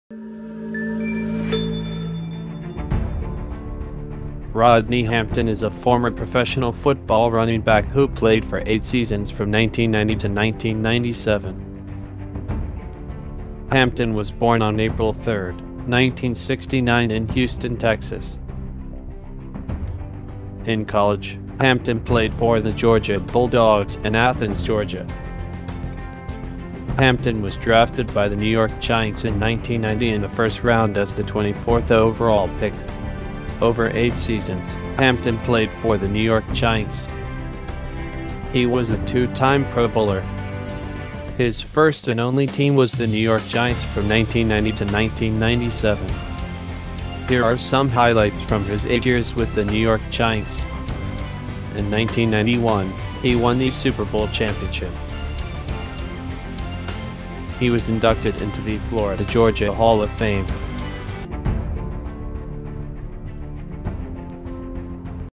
Rodney Hampton: Football Running Back
Rodney Hampton is a former professional football running back who played for 8 seasons from 1990 to 1997.

In college, Hampton played for the Georgia Bulldogs in Athens Georgia.

Professionally, Hampton played for the New York Giants.

He was inducted into the Florida-Georgia Hall of Fame.